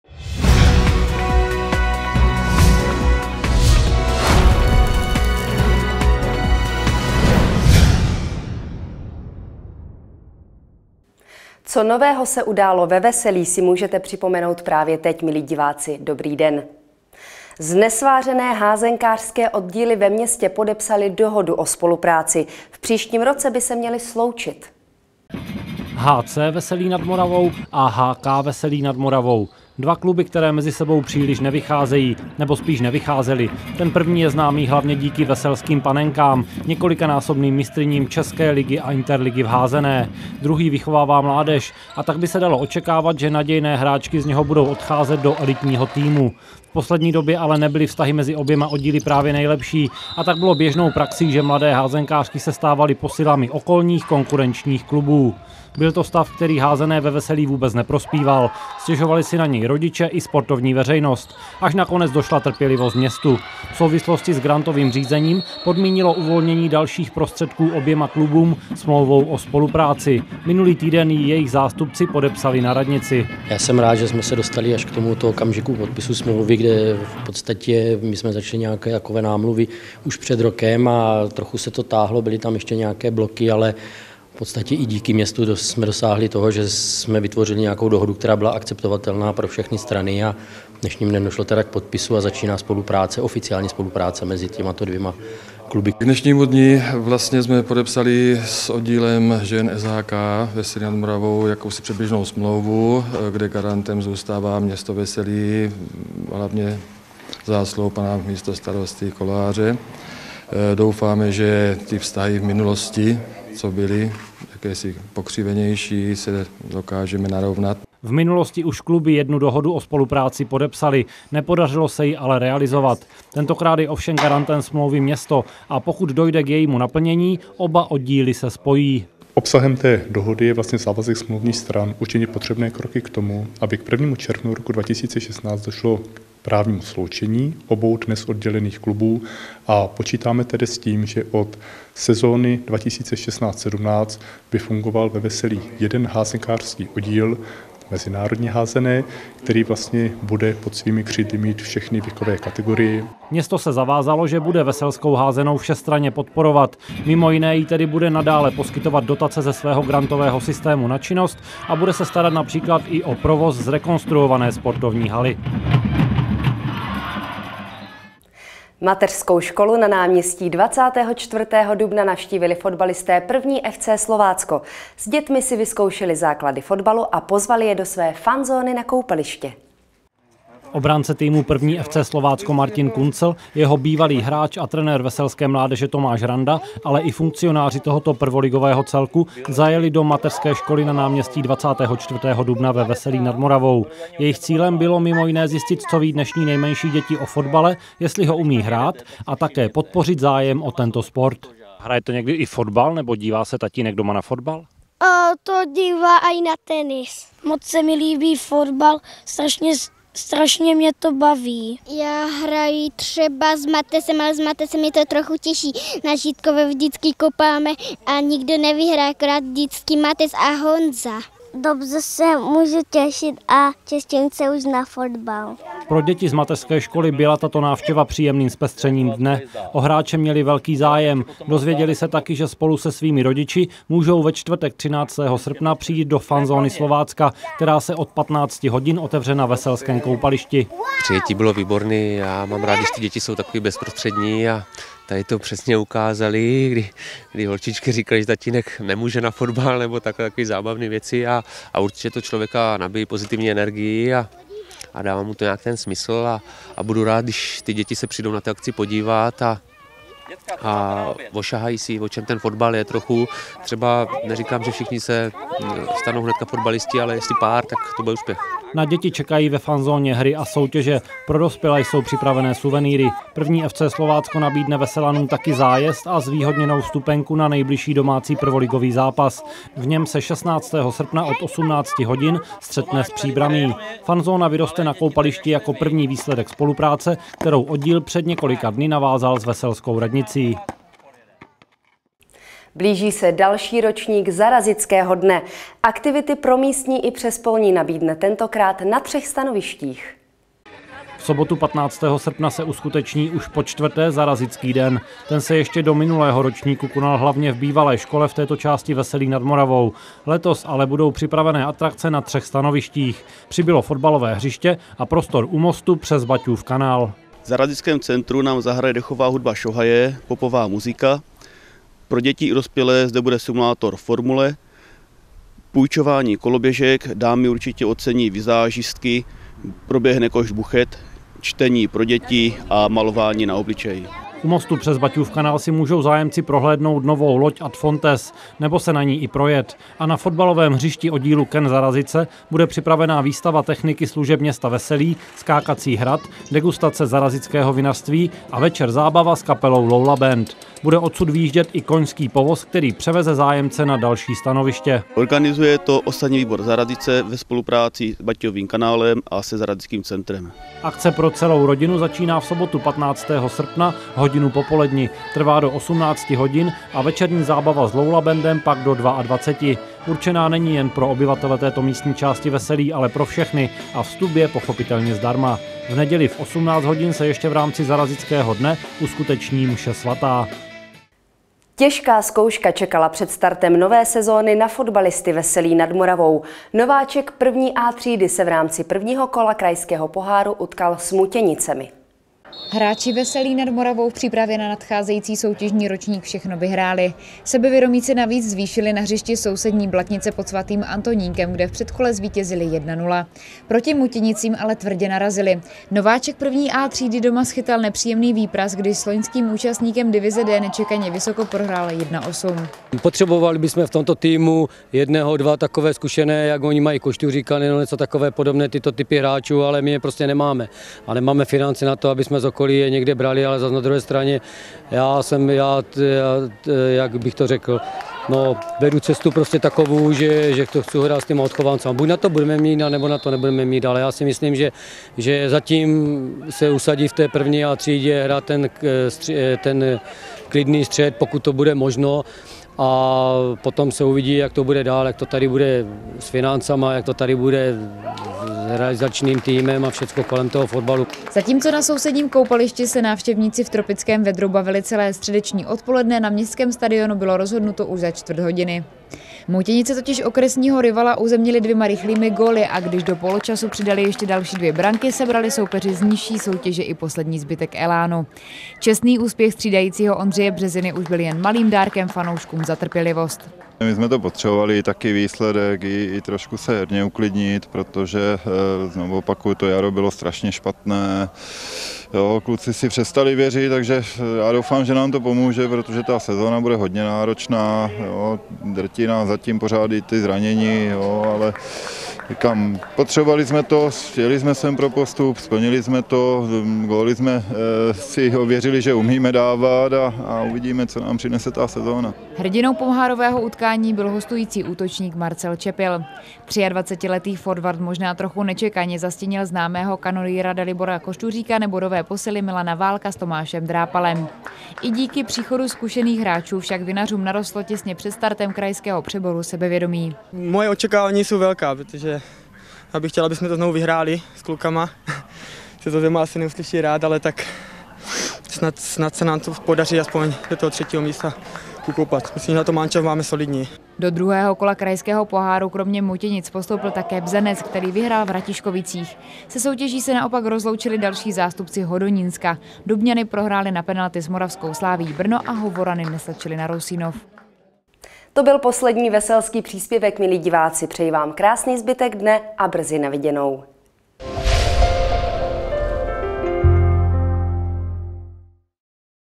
TVS: Veselí 11.8.2015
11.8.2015
Znesvářené házenkářské oddíly podepsaly dohodu o spolupráci / MŠ na náměstí 24. dubna navštívili fotbalisté 1. FC Slovácko / Blíží se další ročník Zarazického dne / Těžká zkouška čekala veselské fotbalisty